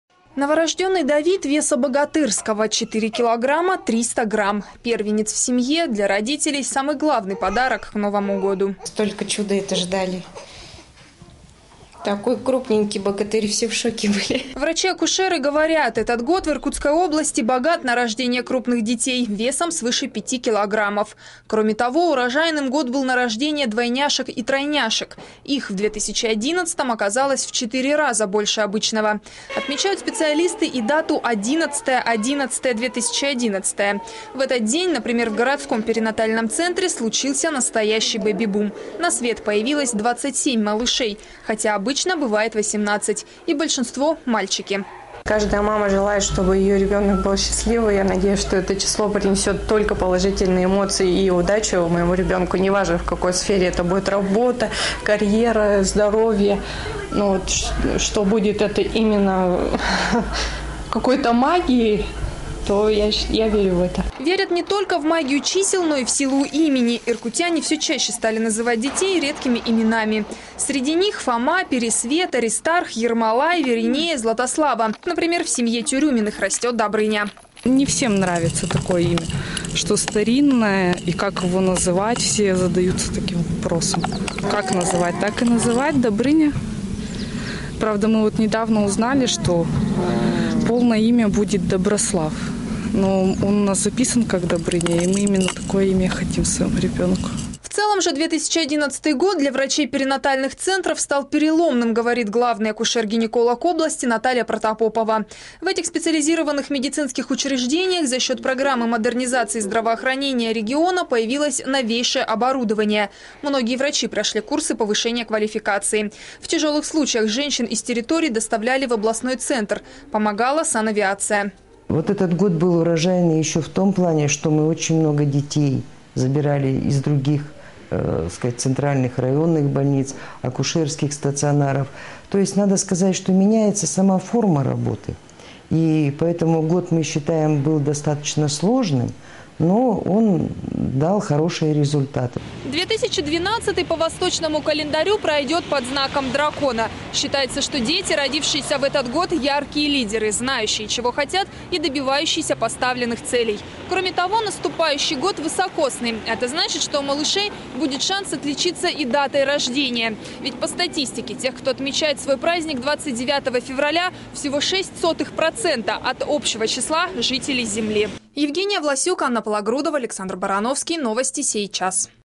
Дети 2011 года
И еще одна социальная мера - с 1 января 2012 года в области вводится региональный материнский капитал. По 100 тыс. рублей получать семьи, в которых появится третий или последующий ребенок. Региональный капитал будет выплачиваться дополнительно к федеральному. К слову сказать, в 2011-ом году в Иркутской области появилось на свет 37 тысяч новорожденных. И как отмечают врачи - акушеры неожиданно много было двое и троен.